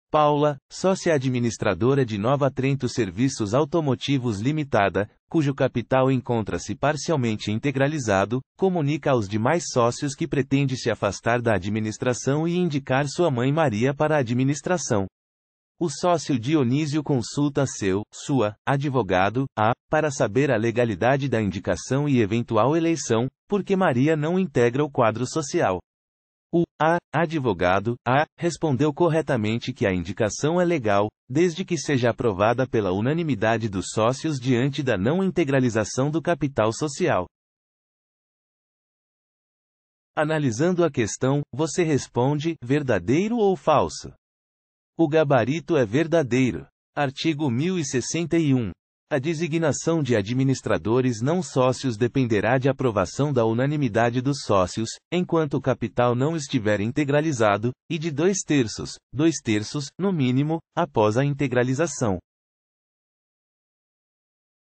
QScon Direito: [Art. 1.061, CC] FGV 2016 - XXI Exame da OAB
Art. 1.061, CC | FGV 2016 | XXI Exame da OAB

Paula, sócia administradora de Nova Trento Serviços Automotivos Ltda., cujo. capital encontra-se parcialmente integralizado, comunica aos demais sócios que. pretende se afastar da administração e indicar sua mãe Maria para a. administração. O sócio Dionísio consulta seu(sua) advogado(a) para saber a. legalidade da indicação e eventual eleição, porque Maria não integra o quadro. social. O(A) advogado(a) respondeu corretamente que a indicação é legal, desde que seja. aprovada pela unanimidade dos sócios diante da não integralização do capital. social.

Gabarito: Verdadeiro